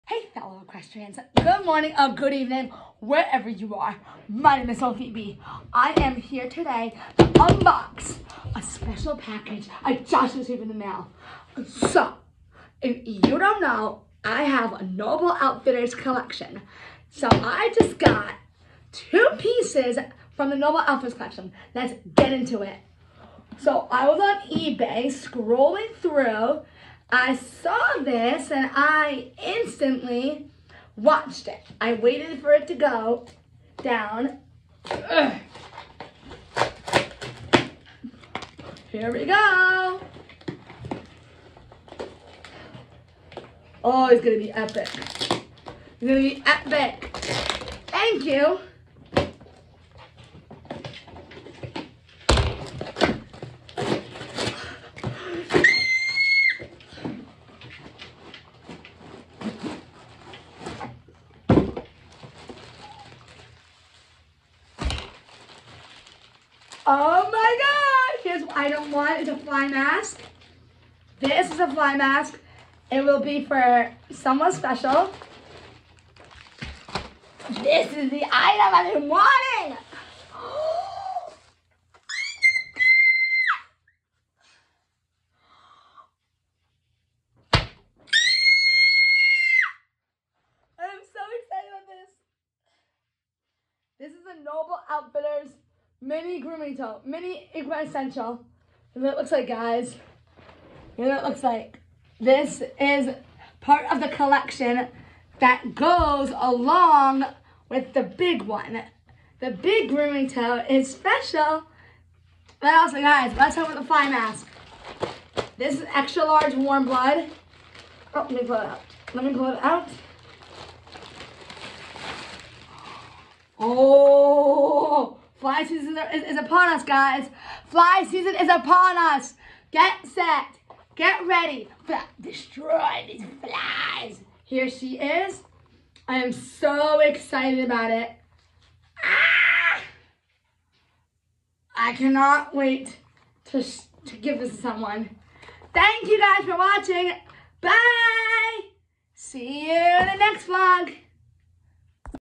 Mini Noble Outfitters Unboxing
Hey there, my fellow horse lovers! How's it going? I hope you're all doing great, whether you're in the morning or evening of your day.

I'm super excited to share with you an amazing shopping experience that I had recently. Guess what? I got my hands on some incredible equestrian products from eBay, and I can't wait to tell you all about them! I'm talking about a fantastic fly mask and a mini equi essential grooming tote from the fantastic brand Noble Outfitters.

You have to hear about Noble Outfitters! This brand has been around since 2011, and their products are just amazing. I can't wait to try them out and let you know what I think.

Okay, let me give you a sneak peek of what you can expect. The fly mask is made of high-quality material that's both durable and comfortable for your horse. It's designed to protect your horse's eyes from flies and other insects while he's out in the field. And guess what? The mini equi essential grooming tote is a must-have for every horse owner. It's a portable grooming kit that contains all the essential tools that you need to keep your horse looking great.

Are you as excited as I am? If you are, then buckle up and join me on this wild ride as I unbox these fantastic products and share my thoughts with you.

Thanks for hanging out with me, and I'll catch you all next time!